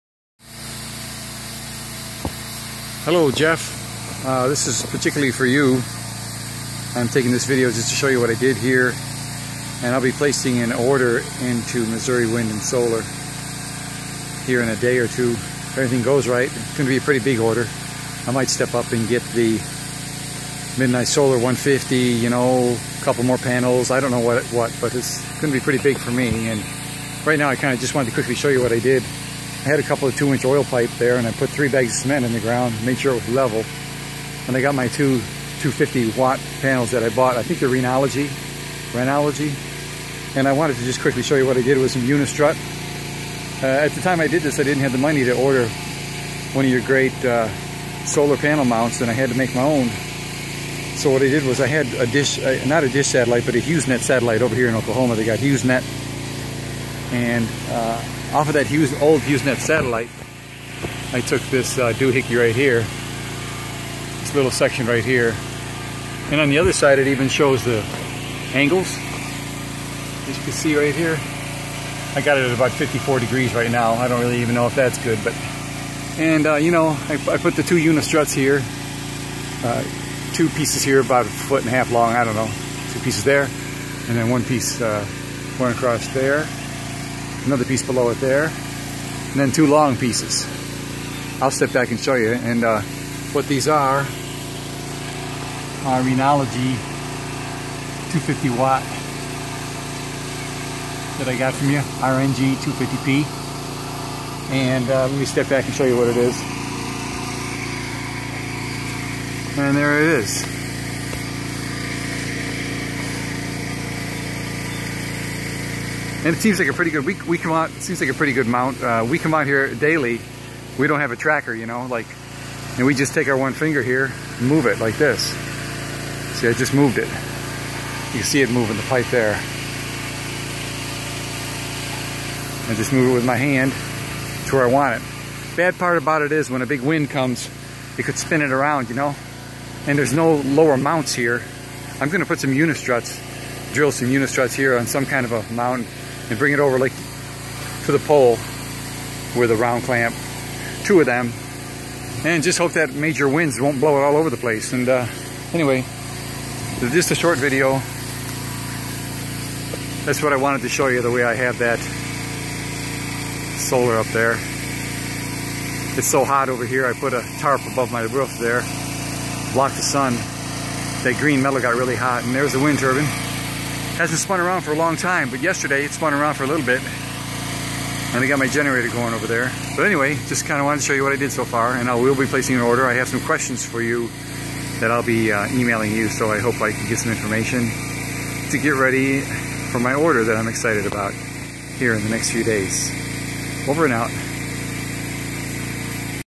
Video for Jeff: Home Made Mount etc.
This video shows my homemade solar mount made out of Unistrut. And also have some questions for Jeff. Go to Missouri Wind and Solar for all your offgrid needs